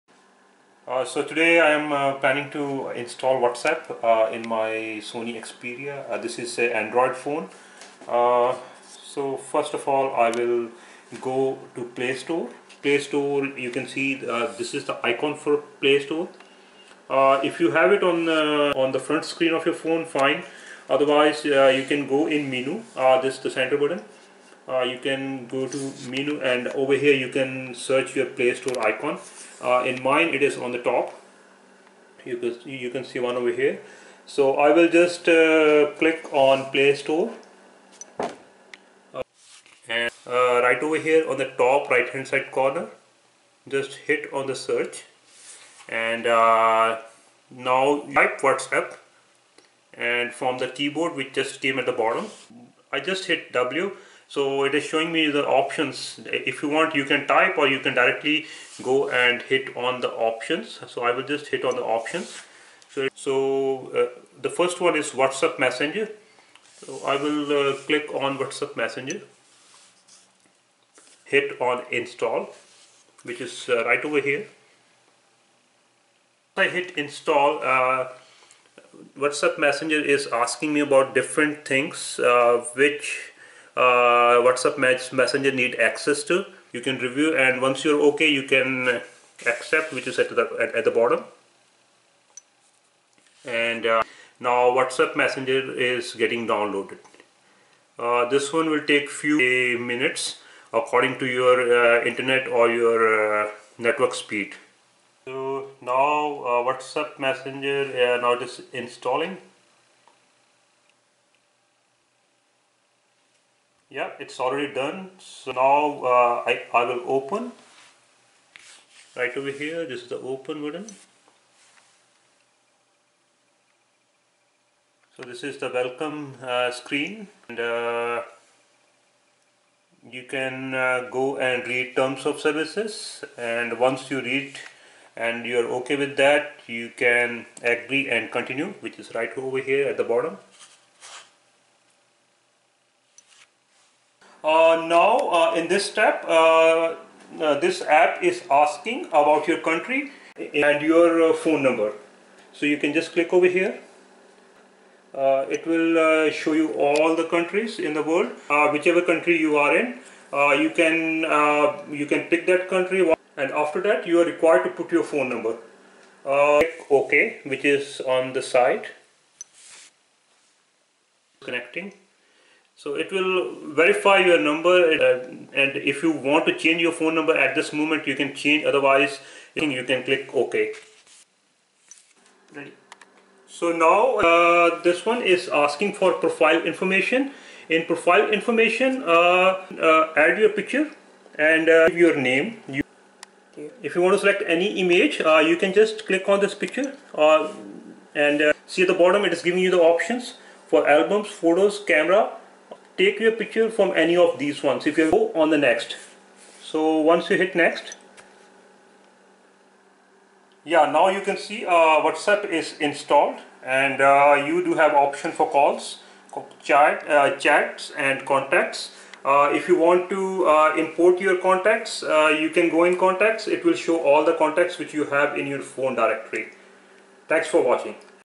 Install Whatsapp Messenger For Android Phone
Download and Setup Whatsapp messenger for android phone.